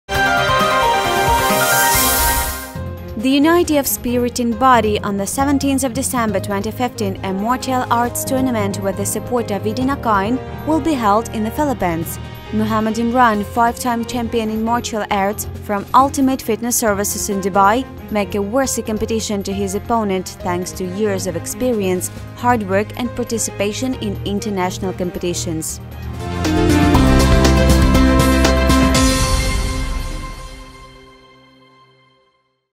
KCN: The martial arts tournament with the support of E-Dinar Coin
The unity of spirit and body. On the 17th of December, 2016 a martial arts tournament with the support of E-Dinar Coin will be held in the Philippines. Muhammad Imran, a five-time champion in martial arts, from Ultimate Fitness Services in Dubai, make a worthy competition to his opponent thanks to years of experience, hard work and participation in international competitions.
EDR: anna-profit
BTC: 1JdRXQuqicwSyn61cd9jyXzAk2QUfu4KEW